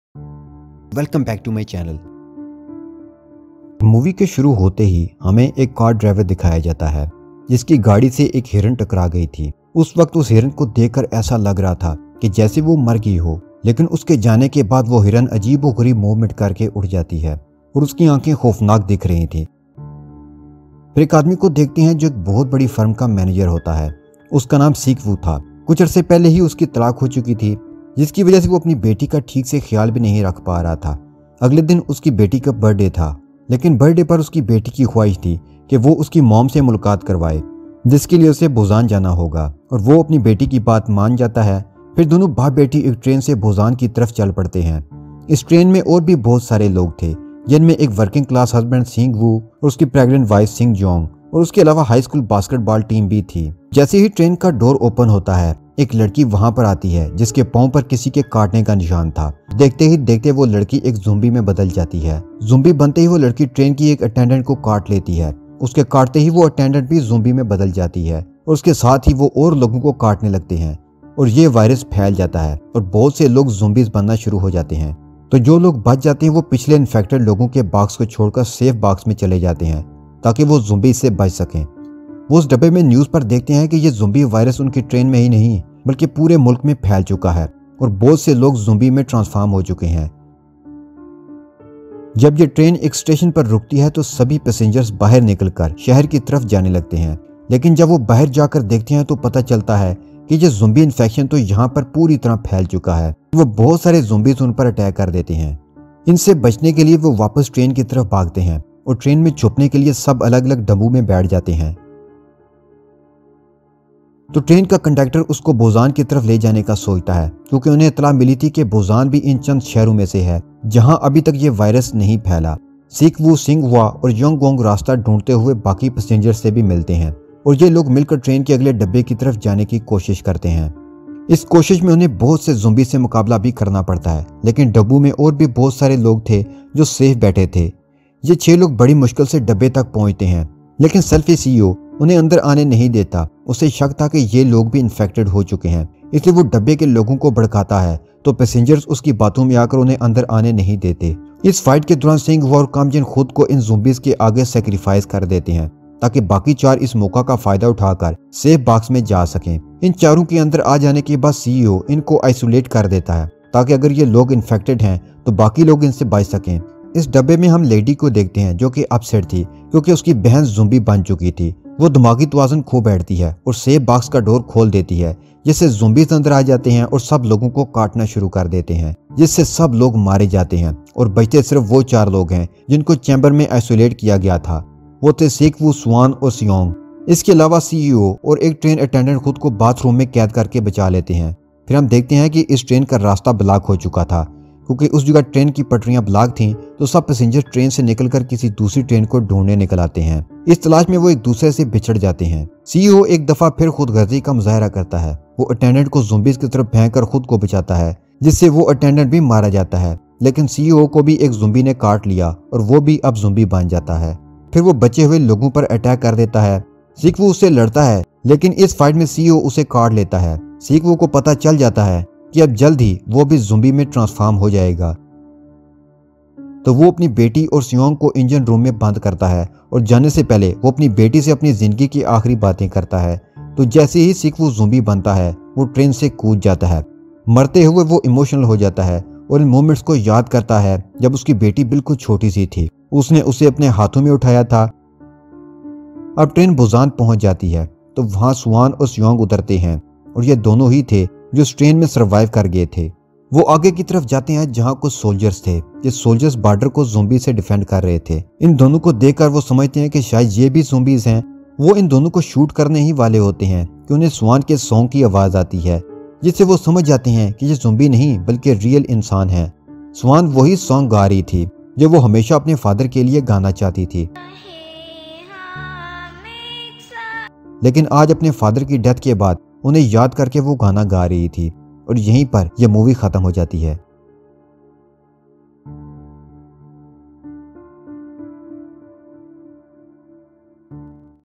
Train to Busan Horror Movie Explained in Hindi and Urdu | Zombie Movie Story Explained
Train to Busan Horror Movie Explained in Hindi and Urdu | Zombie Movie Story Review and Ending Explained